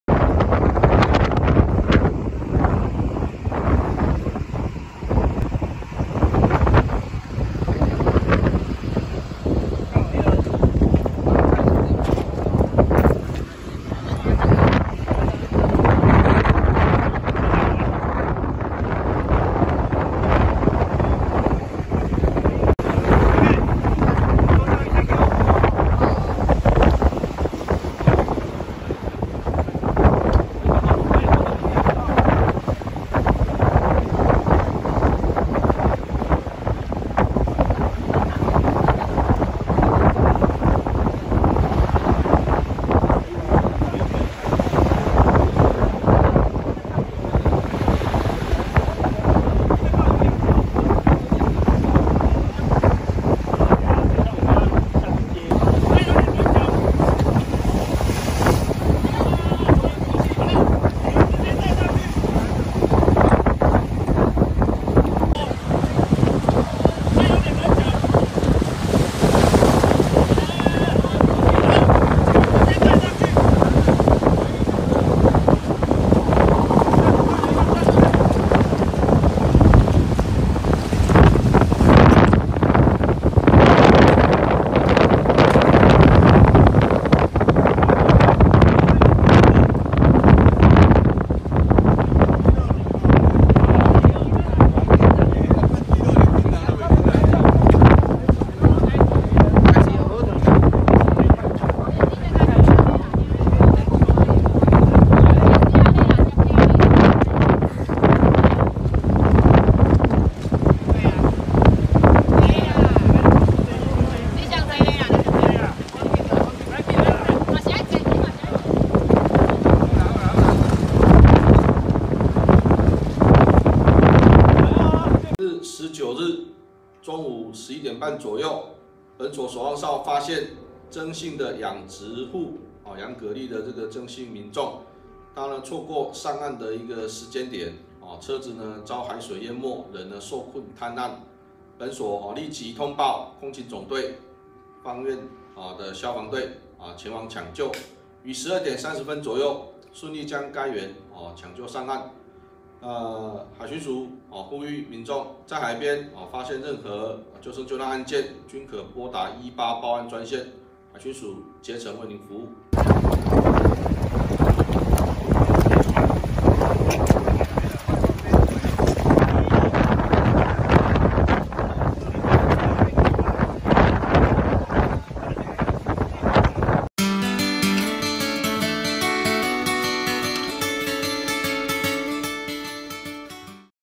彰化老翁受困蚵道險滅頂 海巡警消救援助脫困
〔記者林明佑／芳苑鄉報導〕千鈞一髮！海巡署中部分署第三岸巡隊王功安檢所今(19)日11時40分許漲潮時段，南海尾守望哨人員發現南海尾蚵道有一輛鐵牛車受困，眼看養蛤老翁來不及逃生，險遭滅頂，立即派員攜帶救生裝備並聯繫相關單位前往現場實施救援，所幸海巡、警消人員到場後，成功救援助脫困。